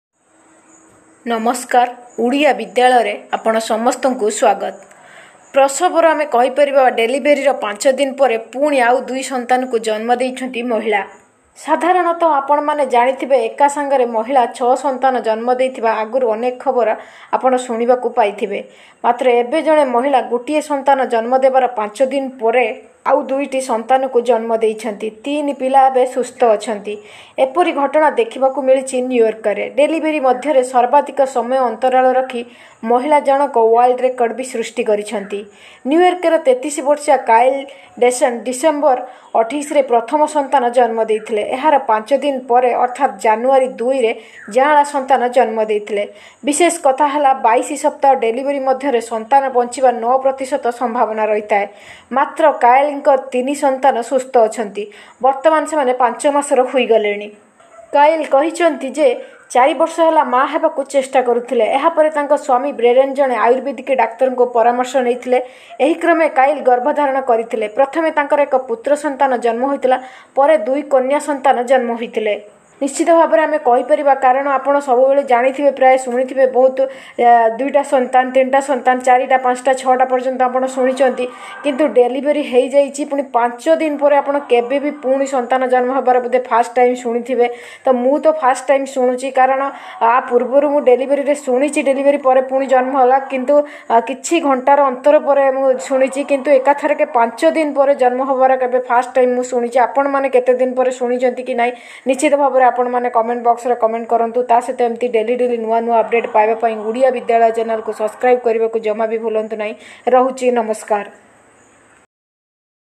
ପ୍ରସବର ପାଞ୍ଚ ଦିନ ପରେ ପୁଣି ଜନ୍ମ କଲେ ଦୁଇଟି ସନ୍ତାନ
odia vidyalaya
odia bidyalaya